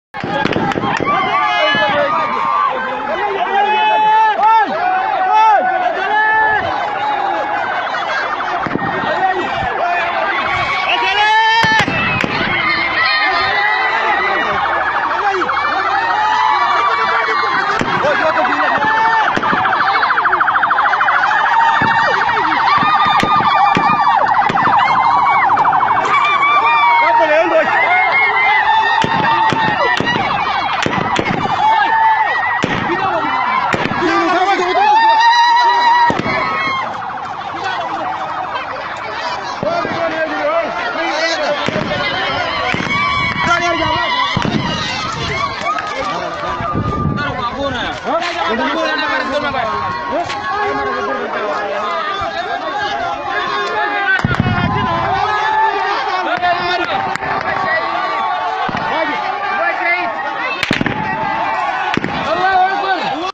Radio Dabanga video: Militiamen open fire at protesters in Darfur camp
The death toll following the incidents at Hasahisa camp in Central Darfur over the weekend has risen to five after a young woman who was shot in the chest by militiamen, died in hospital. In this video of one of the clashes from Saturday January 20, 2018, gunshots can be heard while protesters flee from militiamen.